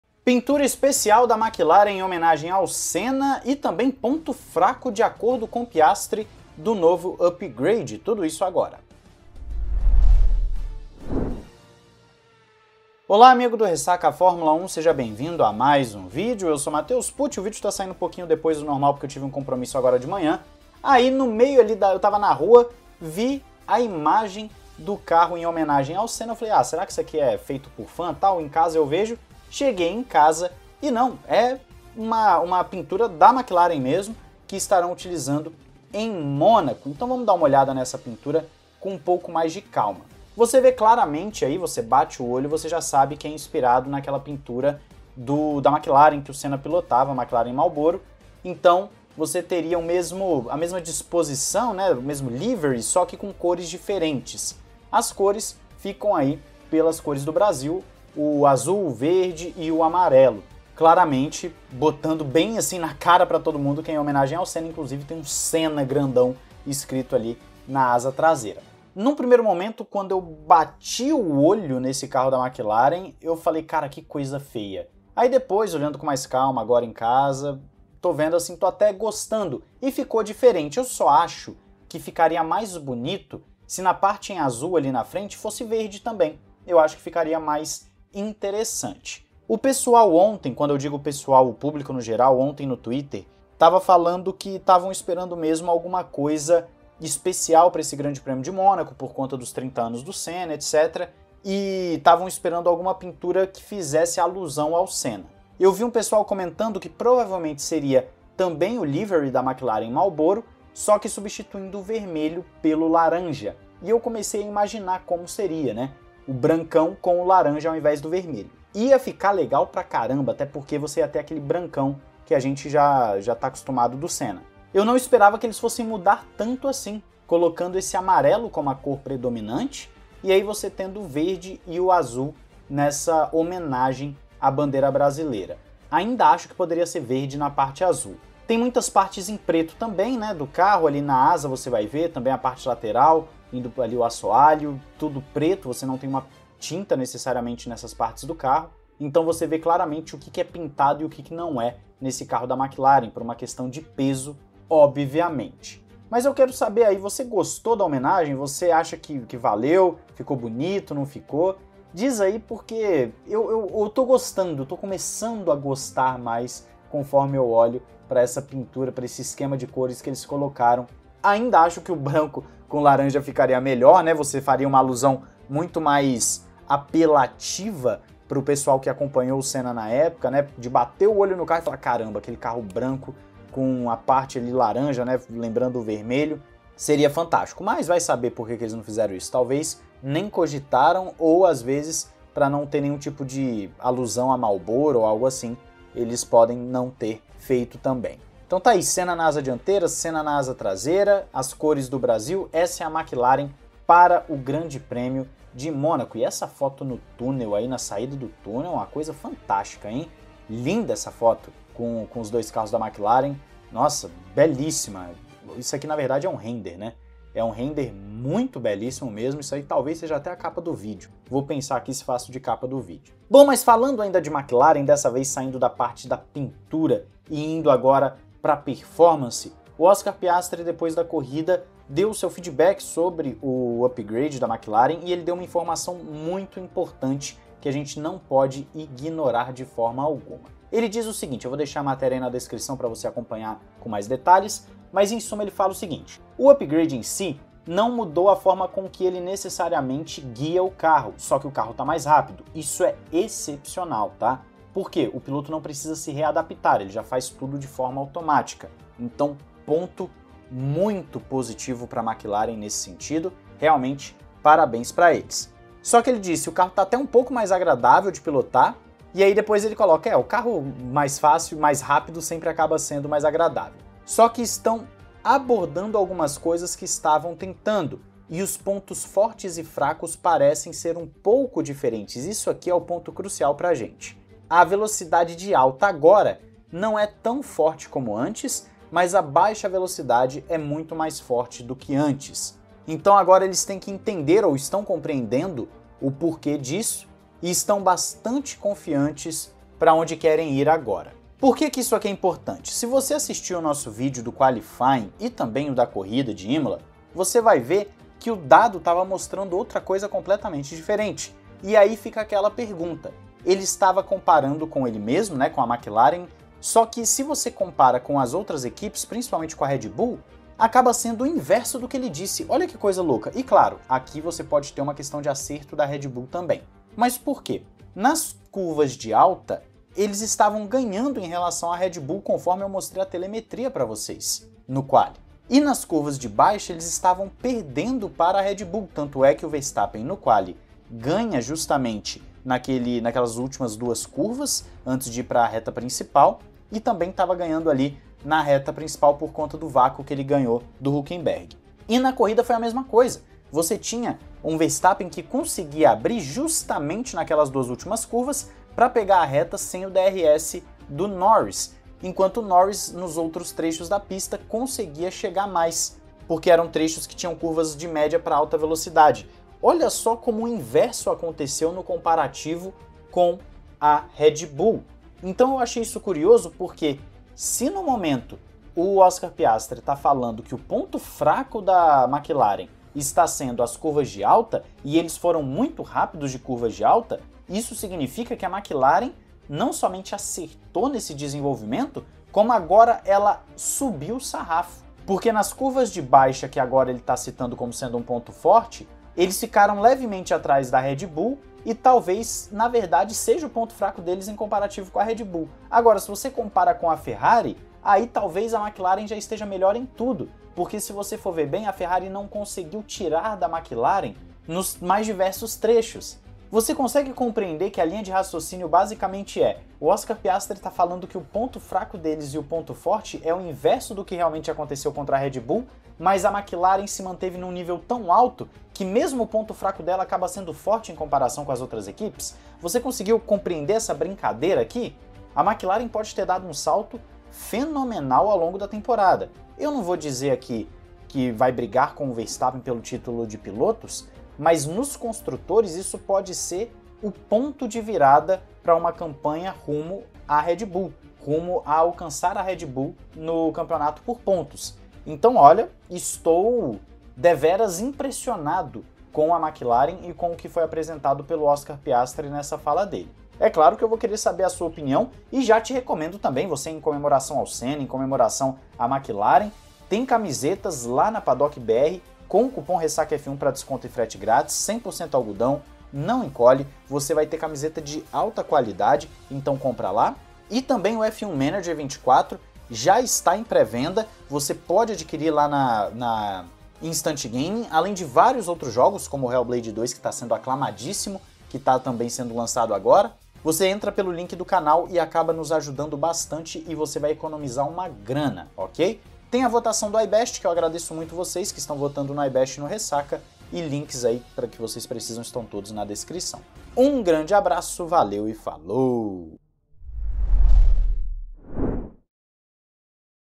MCLAREN TERÁ PINTURA ESPECIAL EM MONACO E PIASTRI REVELA DETALHES IMPORTANTES DO UPGRADE
Sergio Milani
@scuderiamilani (Twitter)

João 3:16

00:00 - INTRODUÇÃO
00:20 - Homenagem ao Senna
03:43 - Piastri pontua mudanças do upgrade
04:44 - Ponto fraco da nova McLaren
06:20 - O ponto fraco está forte...
07:18 - McLaren pode ter dado o grande salto do ano
07:46 - Camisetas SENNA e McLaren
08:00 - F1 Manager e outros lançamentos
08:20 - iBest